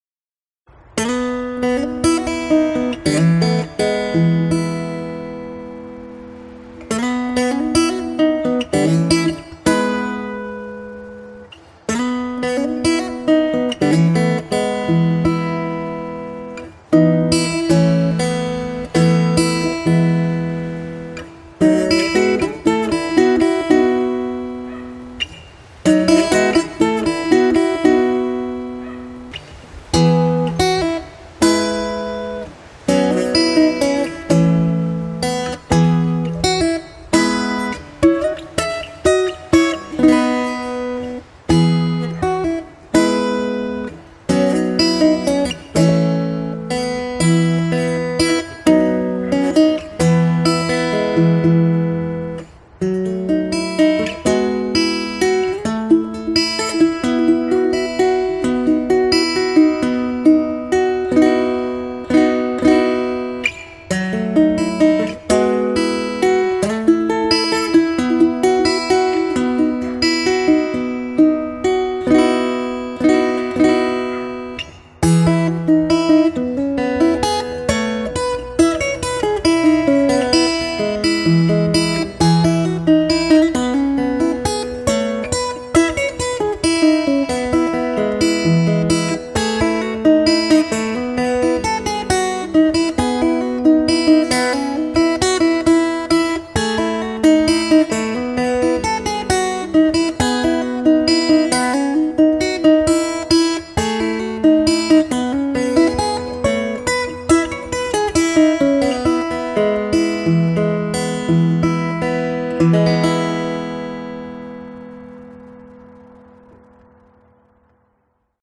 Baritone Ukulele - Finger Style Arrangement
A bit of fingerpicking on my custom designed bari uke by Fred Casey in Canada. It's all solid walnut with a Misi under saddle pickup, and is beautifully constructed with a nice low action. It's a little rare in that I asked for steel strings for a slightly bluesier sound, so Fred has a special design to takes the extra tension on the body. P.S sorry about the dog barking in the background!!